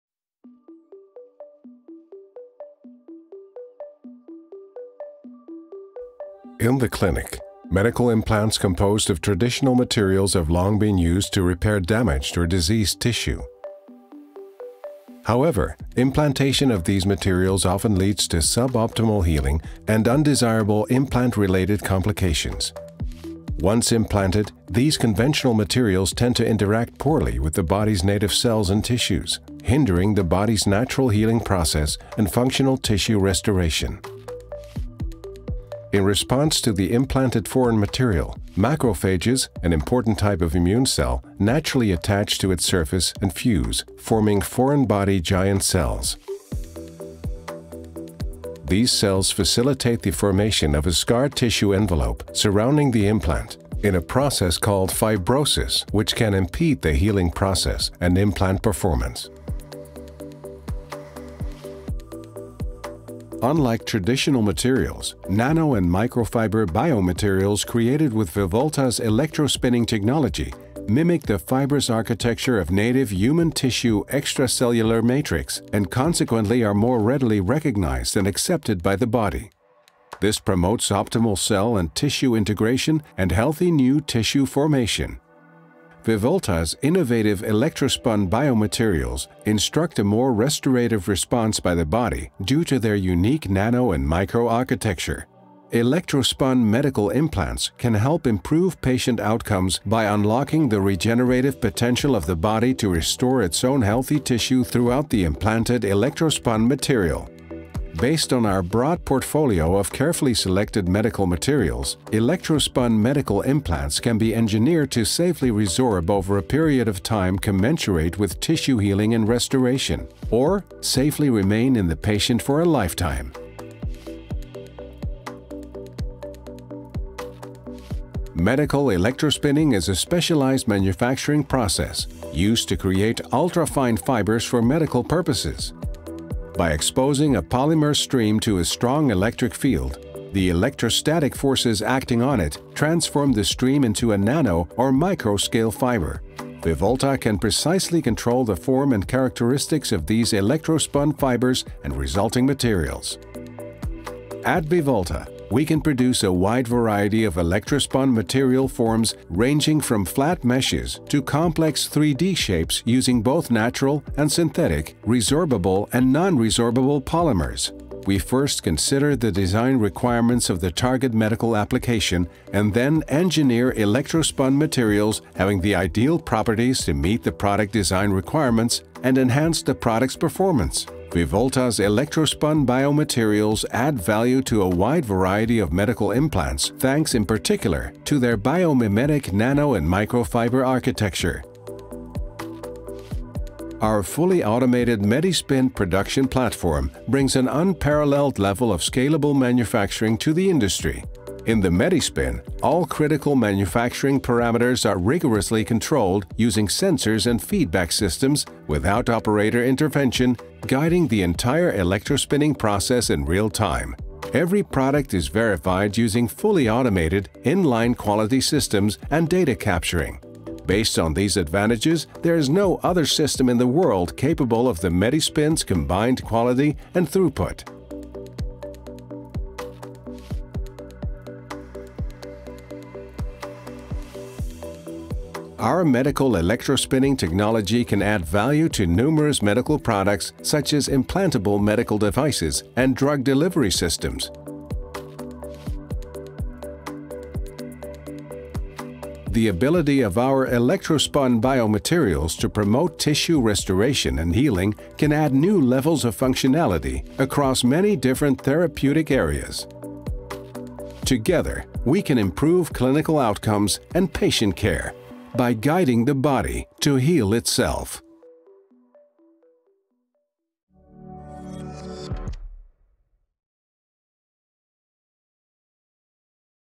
VIVOLTA electrospinning specialists: versatility & benefits of scalable medical electrospinning
VIVOLTA is the medical industry’s trusted partner for contract development and manufacturing of electrospun medical products that guide the body to heal itself.

As a high-performance electrospinning equipment designer and a fully integrated service provider, we enable our partners to develop, scale, and manufacture breakthrough electrospun medical products
that greatly improve patients’ lives.

Our Technology: Medical Electrospinning
Medical electrospinning is a unique manufacturing process capable of producing nano- and micro-fiber-based materials using electrostatic forces. Electrospun biomaterials offer many performance and process advantages over traditional medical materials due to their biomimetic architecture resembling native human extracellular matrix. Whereas traditional medical materials are prone to scar tissue encapsulation and poor tissue integration, VIVOLTA’s electrospun biomaterials are readily recognized by the body, promoting healthy tissue restoration – effectively, guiding the body to heal itself.

Our Solutions: From Concept to Commercial
VIVOLTA offers a full suite of electrospinning solutions spanning from concept development to commercial manufacturing. In development, we first consider the specific design requirements of our clients’ target medical applications, then we engineer electrospun materials with the ideal properties to meet those requirements and enhance product performance.  VIVOLTA’s contract manufacturing services consolidate our clients’ supply chains into a one-stop-shop for electrospun medical products and components – managing raw material vendors and fully integrating post-processes such as laser cutting, packaging, and labelling.

Versatility, Key Applications, & Benefits of Medical Electrospinning
At VIVOLTA, we can produce a wide variety of electrospun material forms ranging from flat meshes, tubes, and even complex 3D shapes using both natural and synthetic, resorbable and biostable polymers. Based on our broad portfolio of carefully selected medical materials, our biomaterials can be engineered to safely resorb over a period of time commensurate with tissue healing and restoration, or safely remain in the patient for a lifetime.

VIVOLTA’s innovative electrospun biomaterials instruct a more restorative response by the body, improving product performance and reducing complications. Our electrospun biomaterials add value to a wide range of
medical applications including cardiovascular, orthopaedic sports medicine, general surgery and soft tissue reconstruction, advanced wound healing, and drug delivery.

The MediSpin Advantage: Unparalleled Quality & Throughput
The MediSpin is the world’s first and only fully automated, scalable medical electrospinning production platform capable of the quality, consistency, and throughput required by the medical industry. This transforms electrospun product concepts into commercial reality.

In the MediSpin, all critical manufacturing parameters are rigorously controlled using sensors and feedback systems, without operator intervention, guiding the entire electrospinning process in real time. Every product is verified using fully-automated, in-line quality systems and data-capturing. Based on these advantages, there is no other system in the world capable of the MediSpin’s combined quality and throughput.